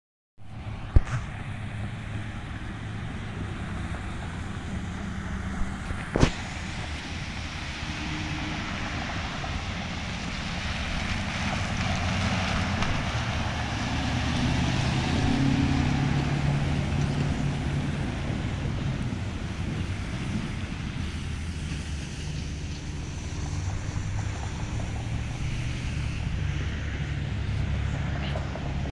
WFD Ambulance 1554 Returning
Waukesha Fire Department Ambulance 1554 Returning From A Medical Assignment
Ambulance 1554: 2021 Ford F550 Braun XL Chief